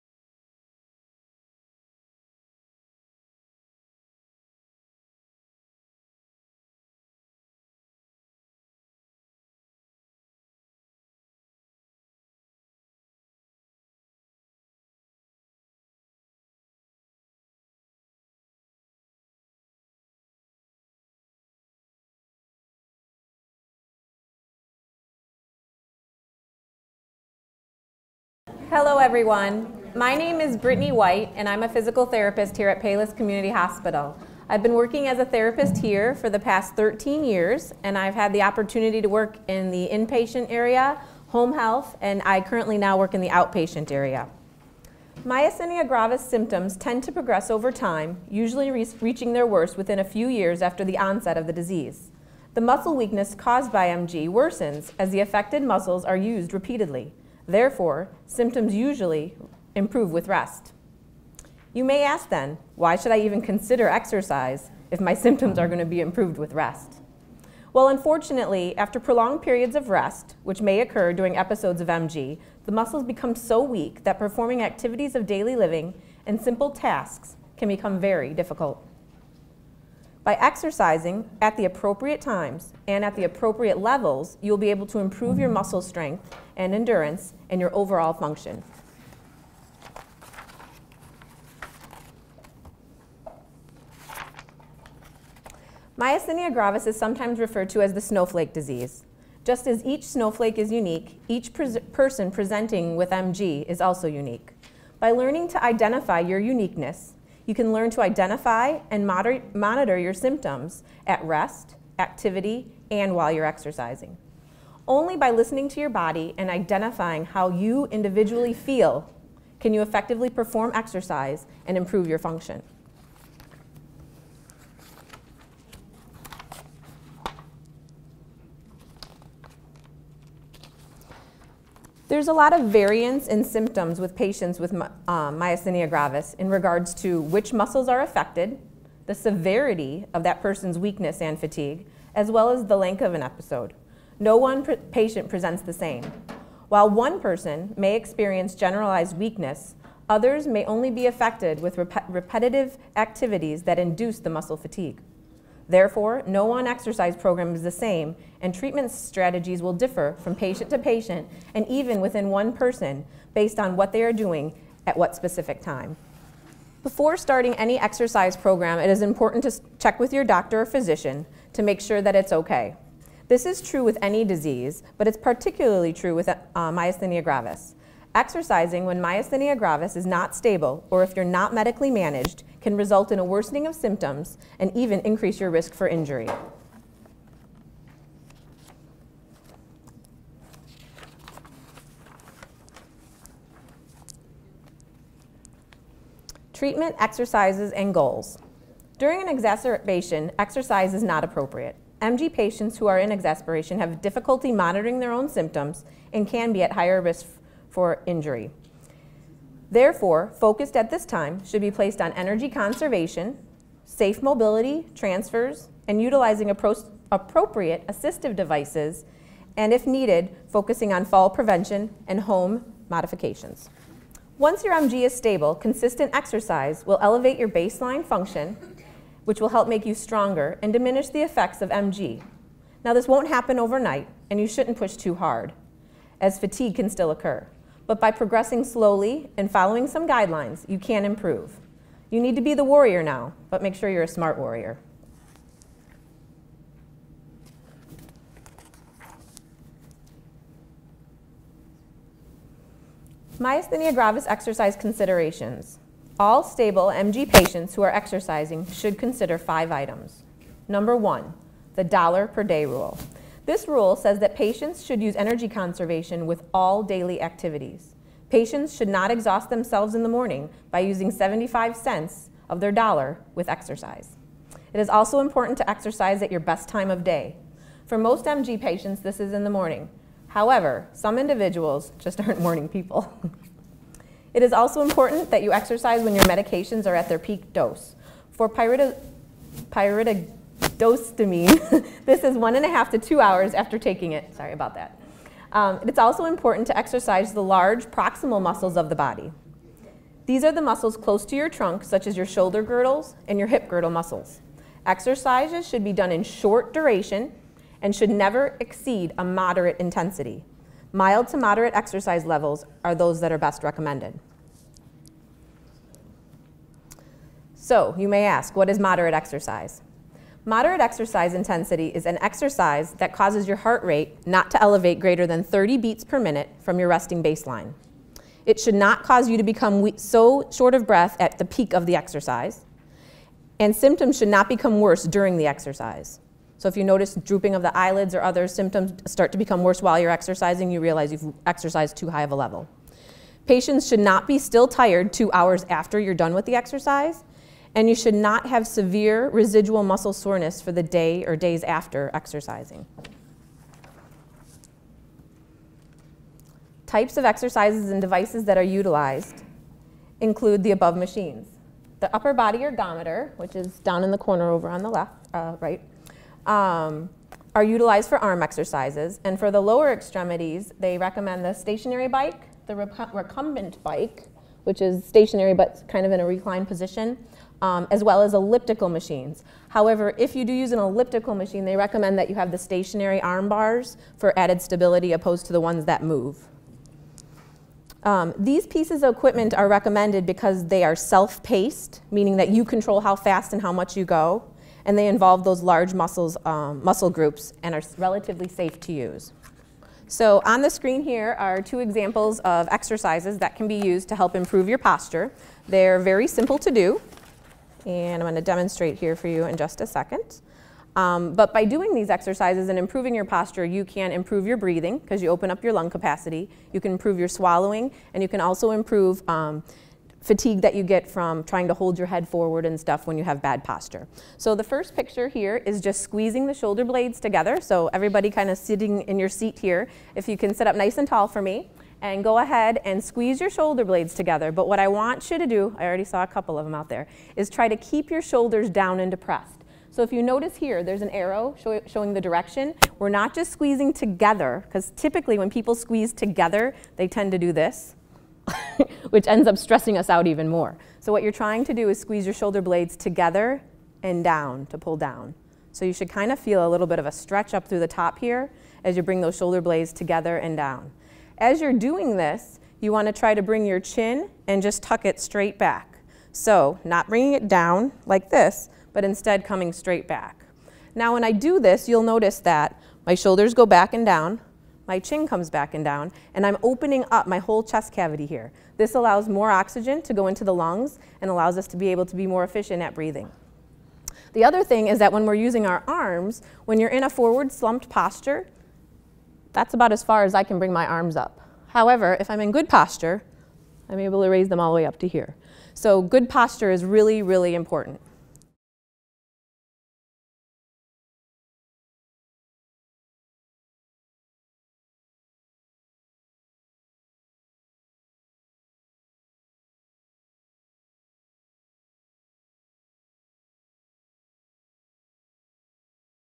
Myasthenia Gravis and Exercise
Physical therapist Brittany White offers exercise guidelines for myasthenia gravis patients. Videotaped at the October 2014 seminar hosted by MGF of Illinois at Palos Community Hospital.